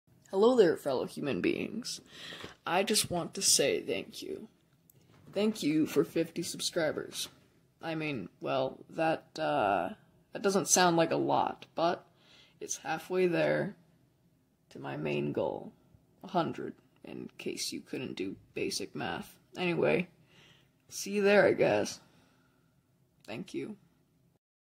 Thank you! (50 sub special)
Thanks so much for half a hundred subs! you all are amazing and you all are litteraly the inspiration for me to animate and continue producing content for you.